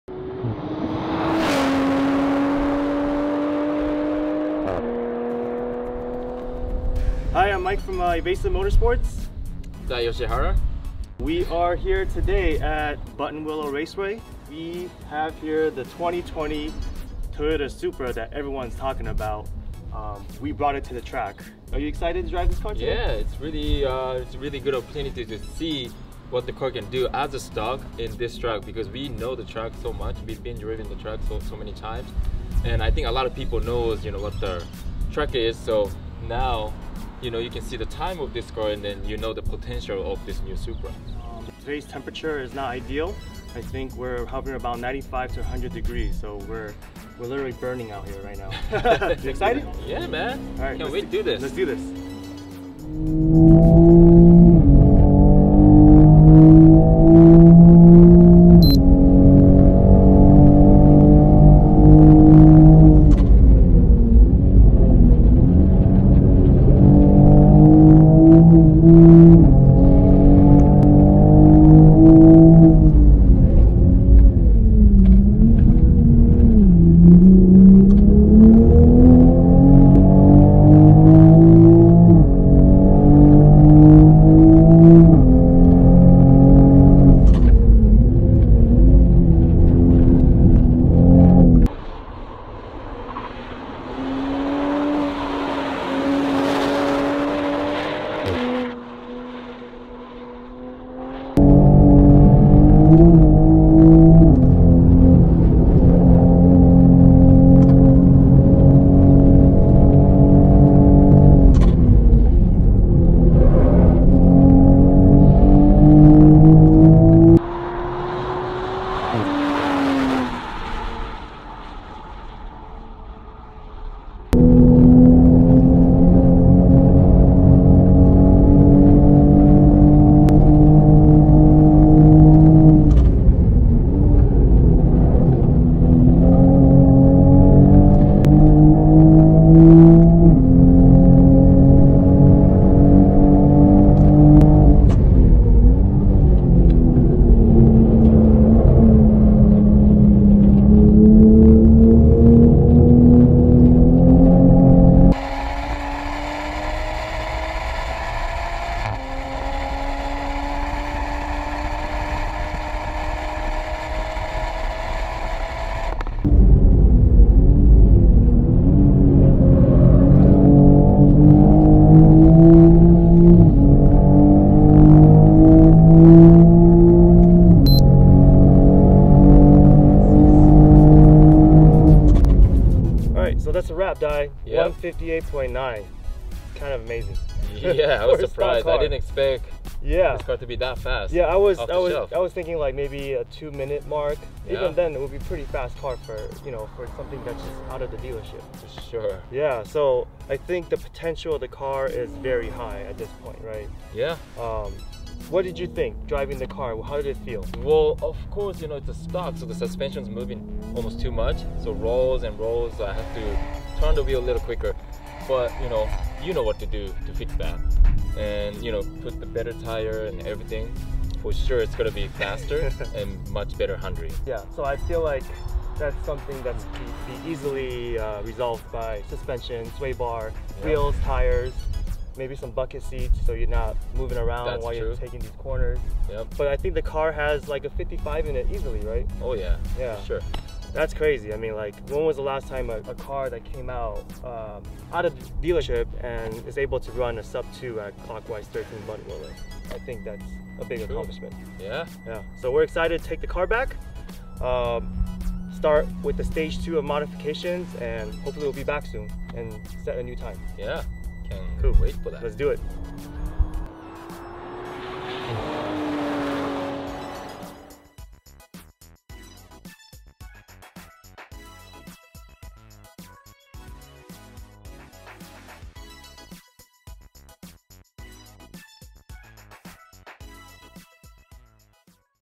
2020 Toyota Supra records first-ever lap time at Buttonwillow Raceway
We took the latest A90 Supra to Buttonwillow Raceway for a quick baseline testing! Formula Drift Champion Dai Yoshihara and Mike Chang from Evasive Motorsports ran a few laps on the California circuit and discussed their overall impression of the Supra as it accomplished a surprisingly quick lap time.

Driver: Dai Yoshihara (@daiyoshihara)
Host: Mike Chang (@mikaljun)
Editor: Roy Fann (@royfanntl)